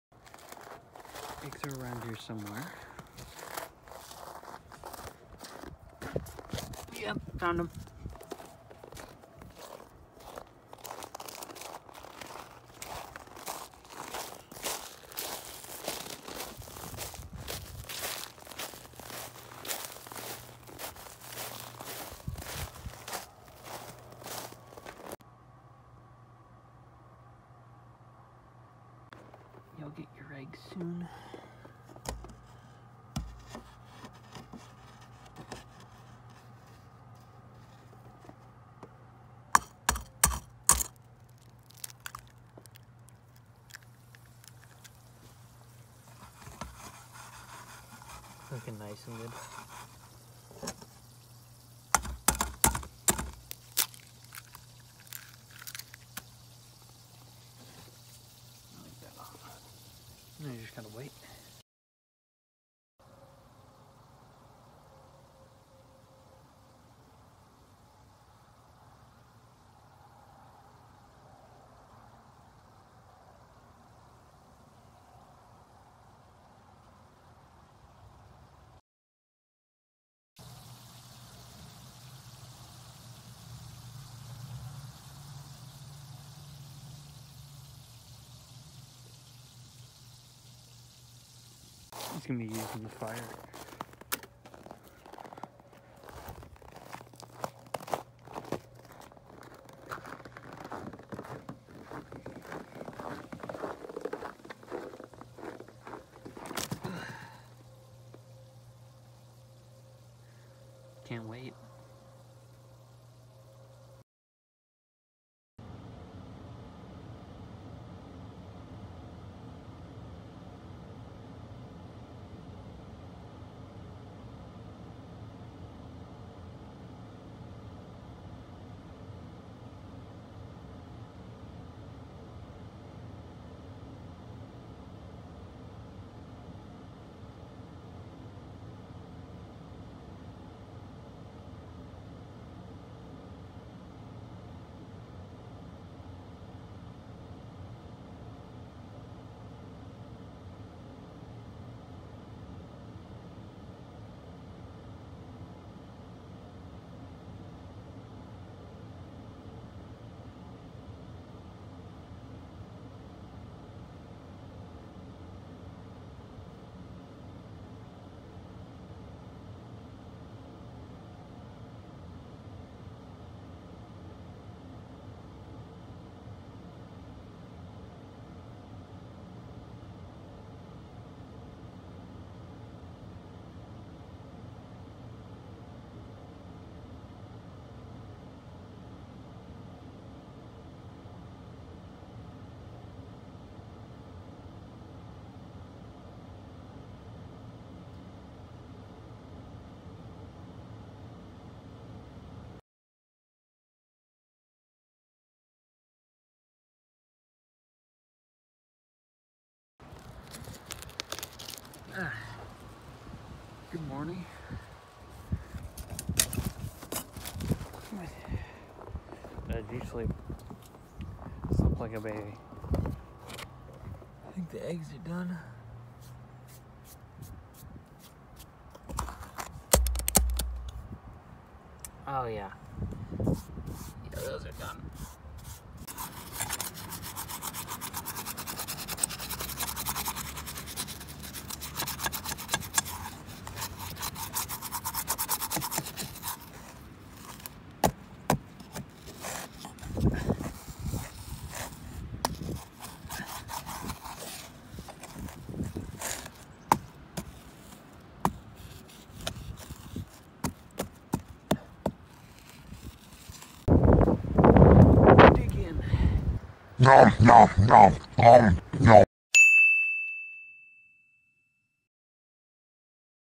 Making Eggs: Mr. C The Female Sloth
Me and Mr. C worked up quite the appetite so I decided to fry up some ol’ fashioned eggs!